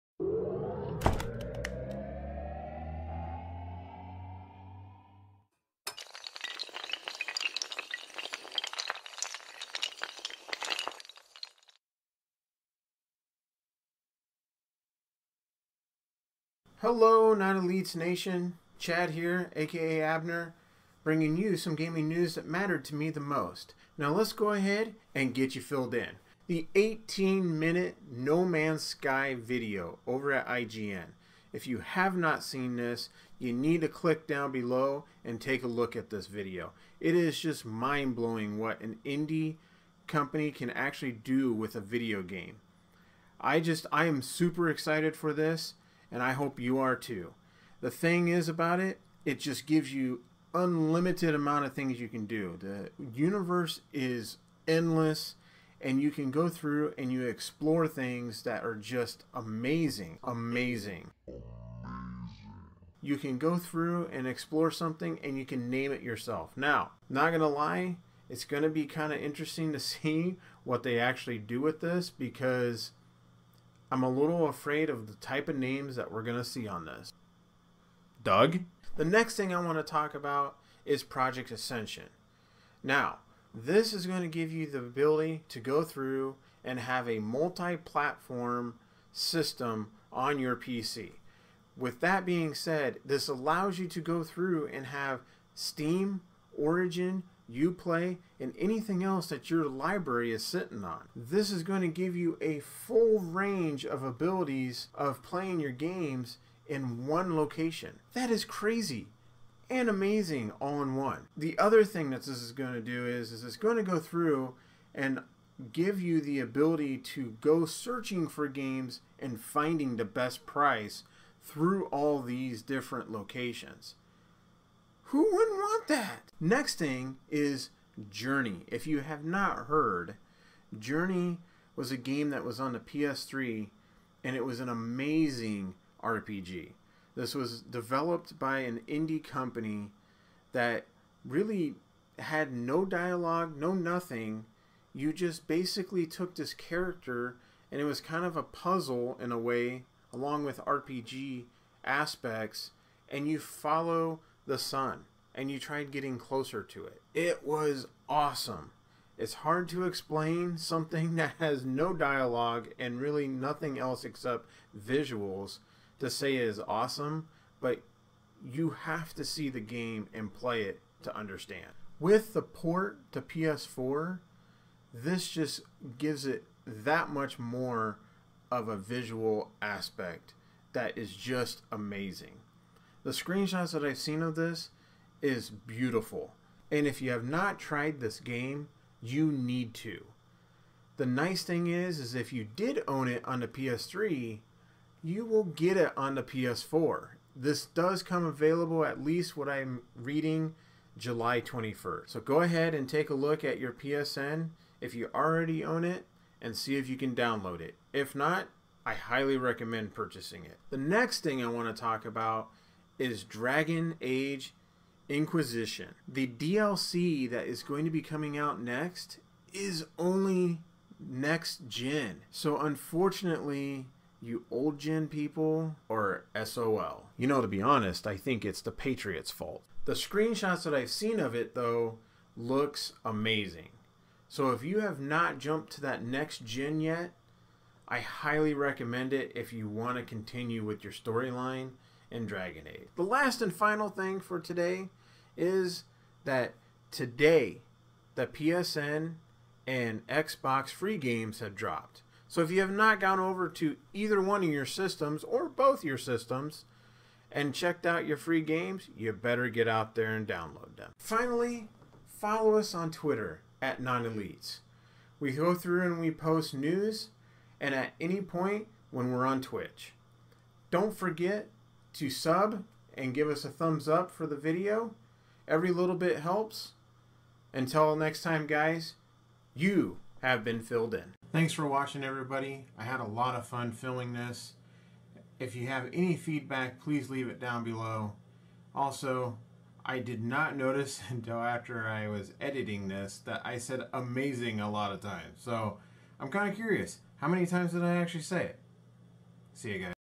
Gaming Vlog w/Chad talking No Man's Sky, Project Ascension and more
My first Gaming Vlog and bringing you some gaming news. Please check out the links on some of the stuff I talk about in the video. Thanks

Your Gamer corner for all your Game News, Gameplay, Game Reviews and Gaming Podcast.